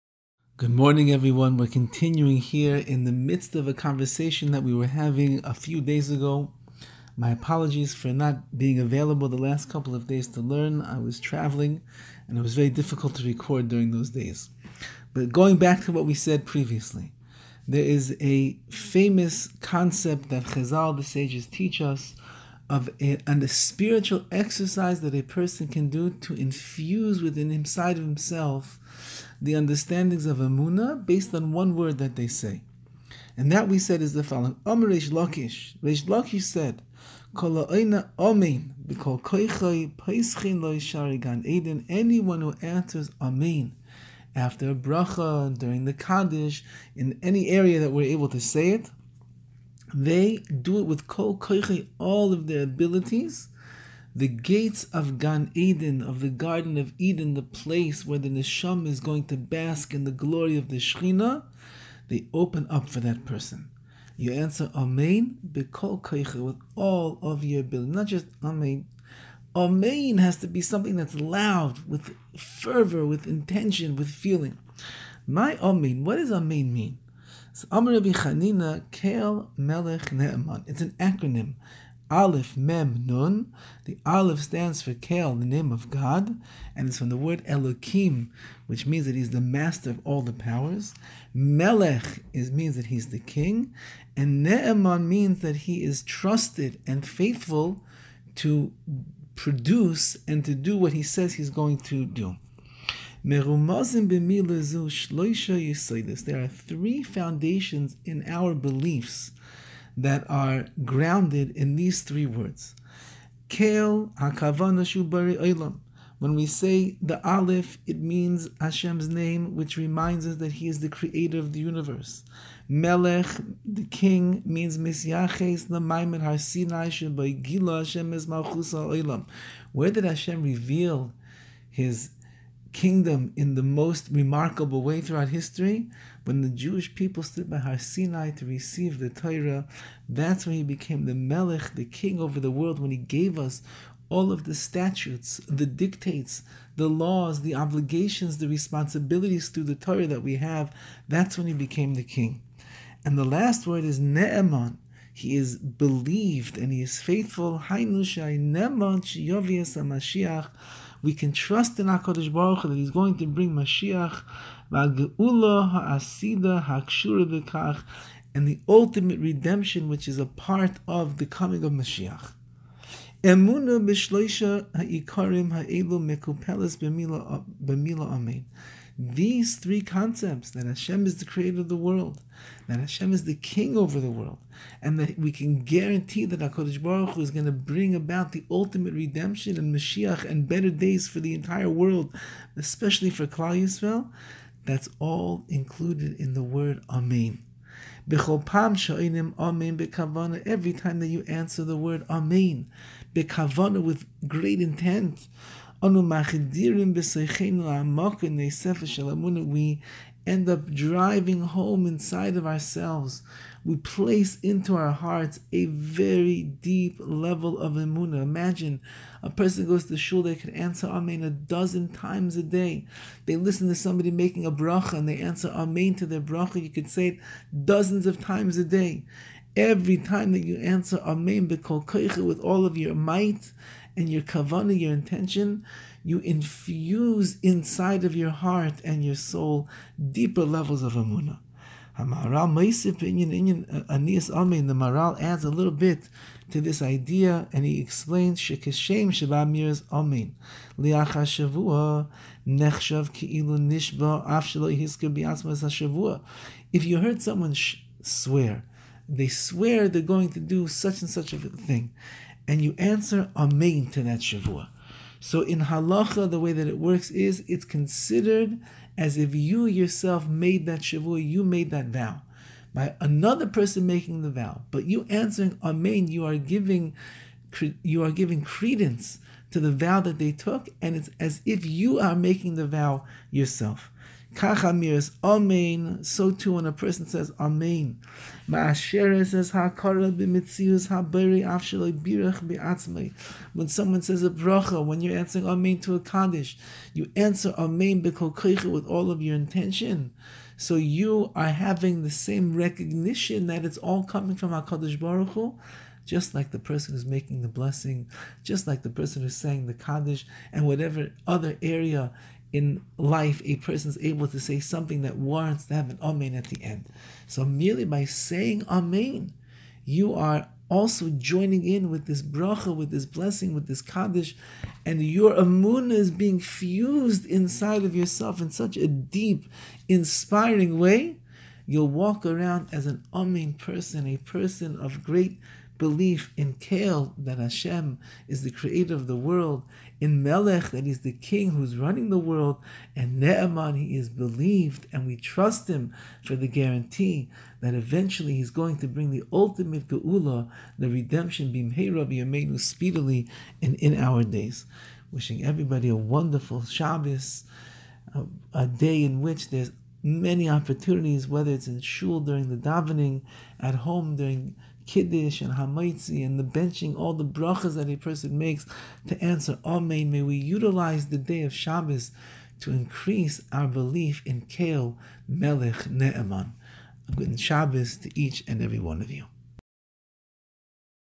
086_The_Power_of_Amein
"With Emunah You Shall Live", Rav Shlomo Wolbe book - Series by Rabbi Dovid Horowitz  שליט׳א